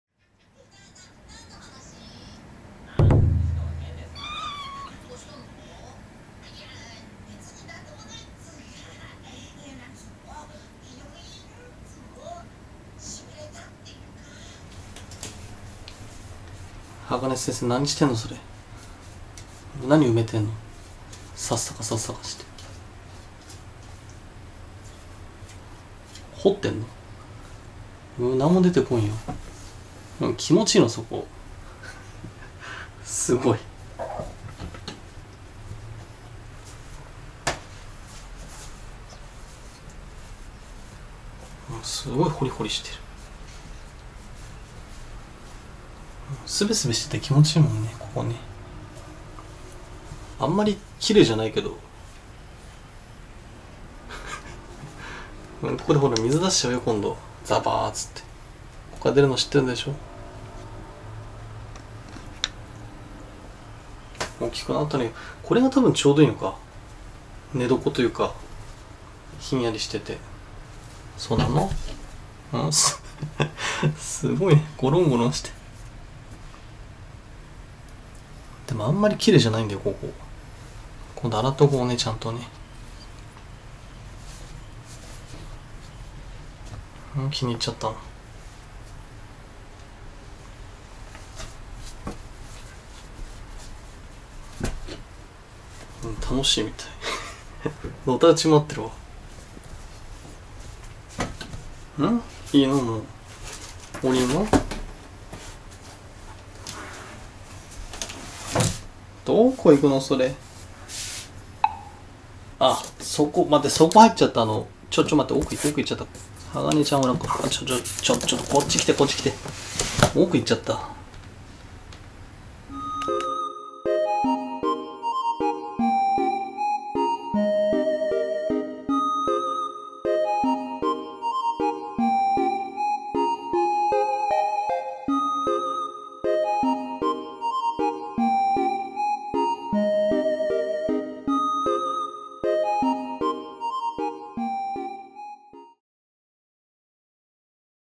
【正式名称は「洗髪洗面化粧台」】すべすべ空間に夢中でのたうちまわる子猫【はがね先生】
■あいろんTwitter： (もしくは「あいろん」でTwitter内検索をお願いします)
サイベリアンフォレストキャット（Siberian）の子猫を、2017年7月1日に新たに家族に迎え入れました。猫の中でも特に賢く、運動量が多く、また大型になるというサイベリアン。「はがね先生」との毎日を、動画として残して行こうと思います。